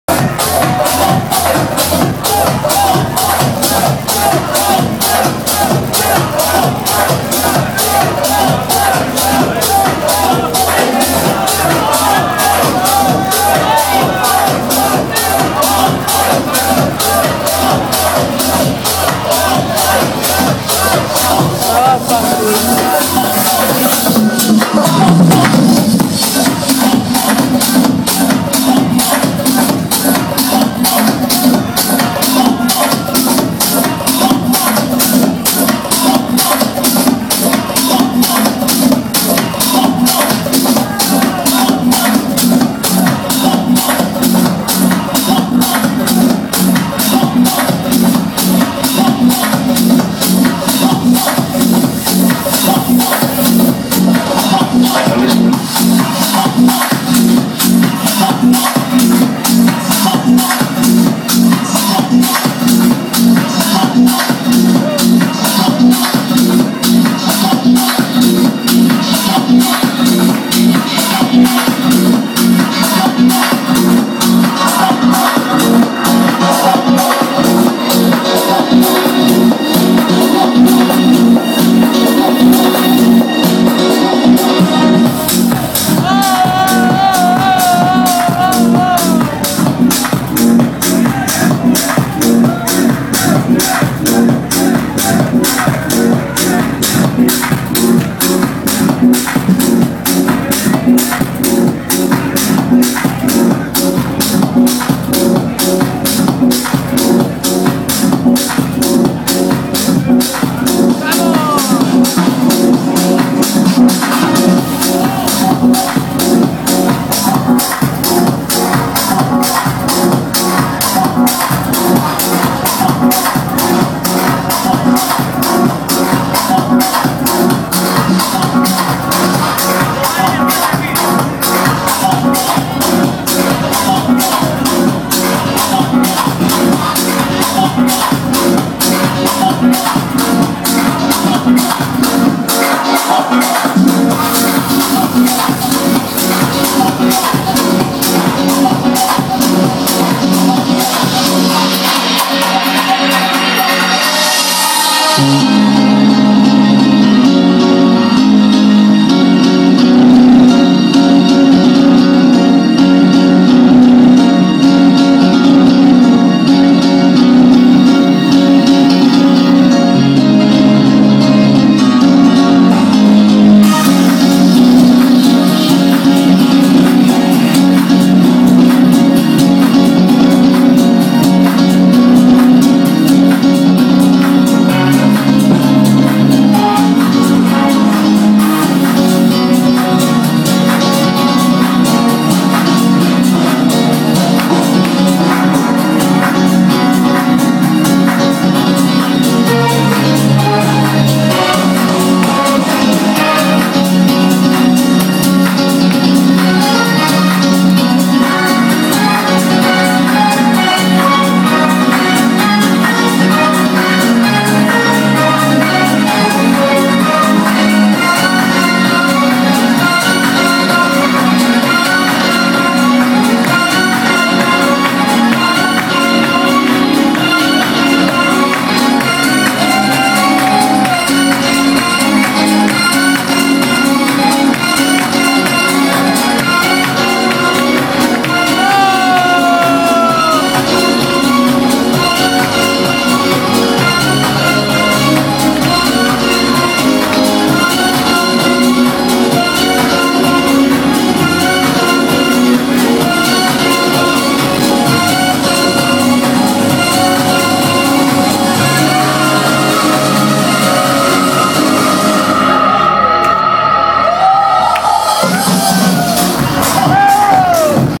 Sergio Dalhbak @ Cocoloco (Coalition, Brighton, UK) 11/01/2012
First cocoloco party of 2012 season... featuring special guest "gogo malaguita&friends dancers"